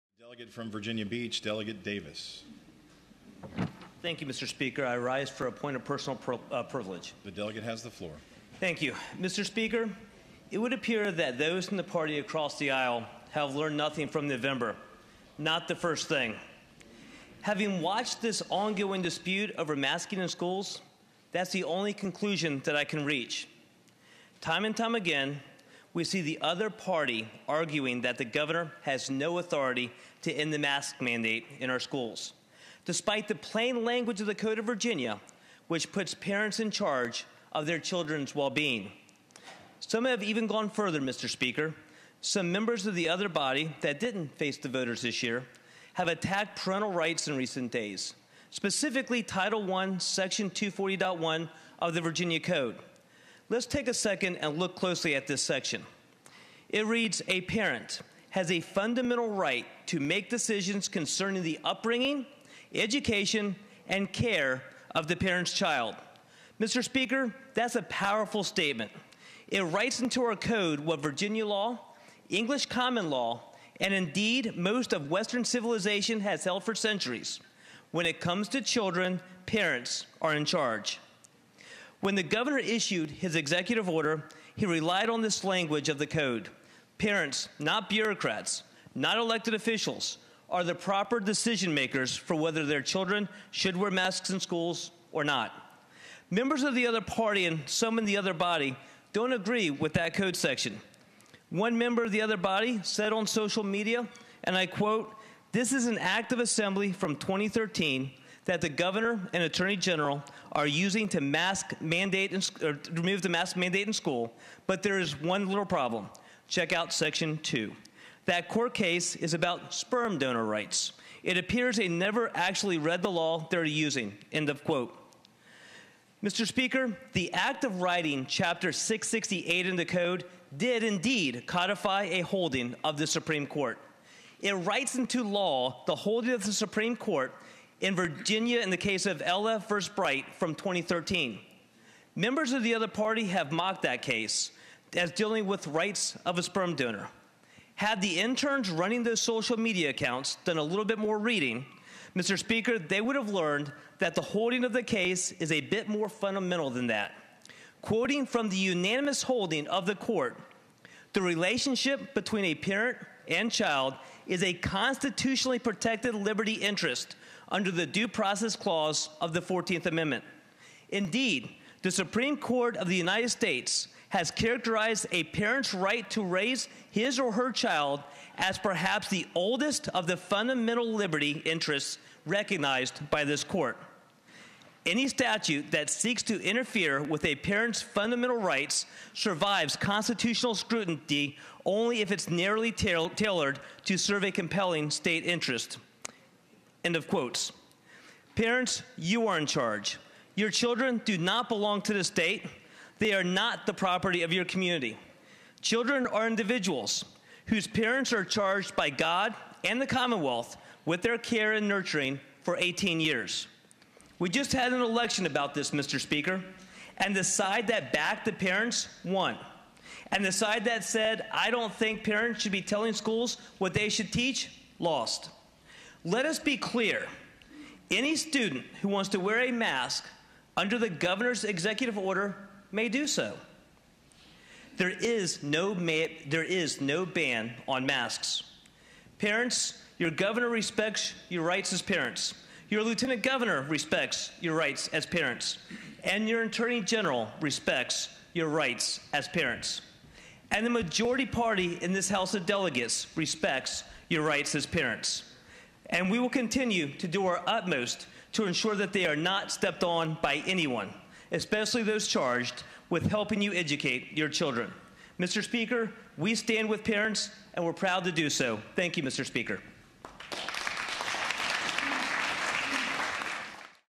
Del. Glenn Davis: Republicans Stand with Parents
Del. Glenn Davis, R-Virginia Beach, tells the House of Delegates in no uncertain terms that Republicans stand with parents on their fundamental rights to raise their children, especially on issues of masks.